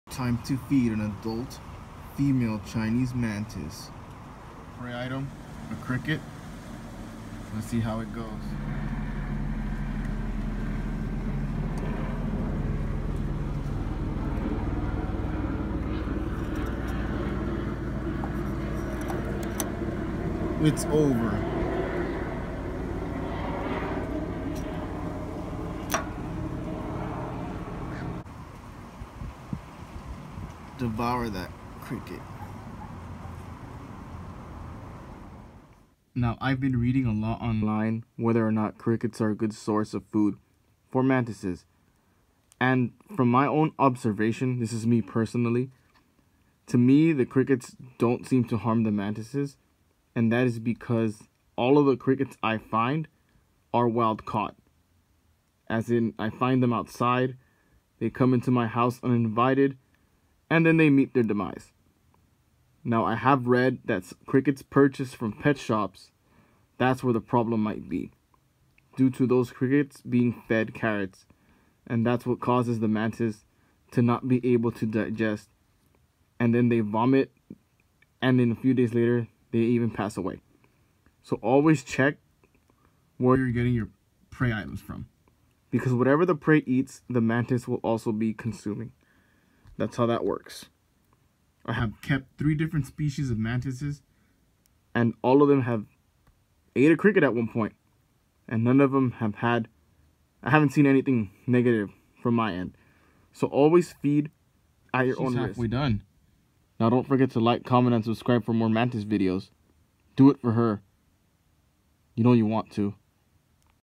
Mantis Care #4 - Feeding Adult Mantises, Mantis eats Cricket
Feeding adult mantises is exciting. As adults, mantises are now prime hunters and are able to take down larger prey. Roaches, spiders, mealworms, superworms, etc are all excellent candidates. When handling prey insects; yes, they will become food but do not mistreat them.

All the prey insects I feed my mantises are wild caught. Throughout the years I have spent in this hobby; crickets are the one insect, people tend to avoid. Crickets can harbor bacteria and viruses that do not affect reptiles and amphibians. Crickets from pet shops are fed carrots; and that causes the mantis to vomit or even die in a few days. Even knowing this; I have not experienced any negative side effects when feeding crickets. I have worked with these four species of mantises: Tenodera sinensis, Stagmomantis limbata, Mantis religiosa, and Iris oratoria. I saw no negative reactions from feeding them crickets. If you have exotic mantises; please take precaution or let me know your results.